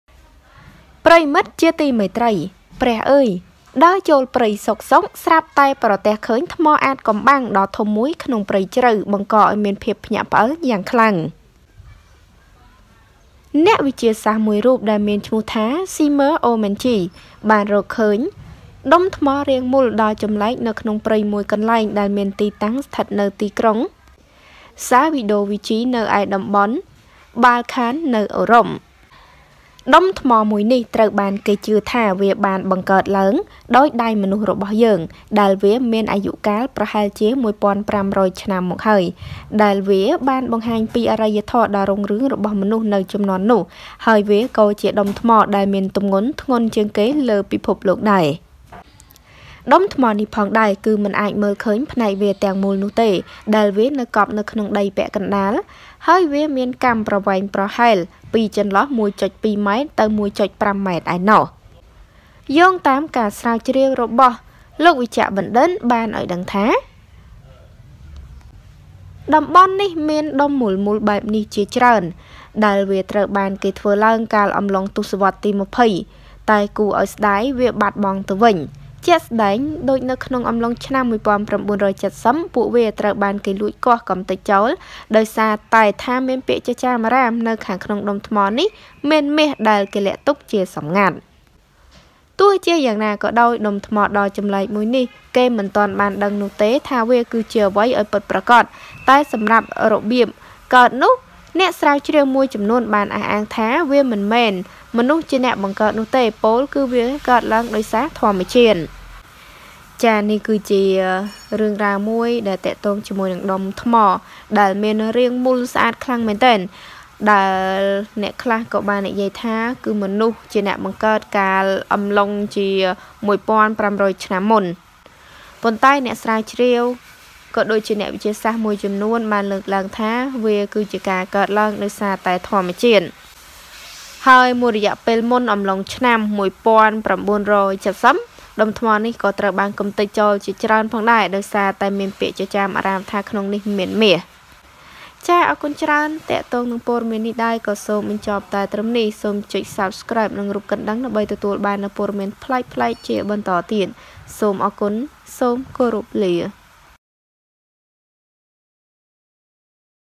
ព្រះអើយ!! ដើរចូលព្រៃសុខៗ ស្រាប់តែប្រទះឃើញថ្មអាថ៌កំបាំងដ៏ធំមួយក្នុងព្រៃជ្រៅ បង្កឲ្យមានភាពភ្ញាក់ផ្អើល
សូមអធ្យាស្រ័យ

ចំពោះការមិនបានតប ខំមិនបានគ្រប់គ្នា

Ms Socheata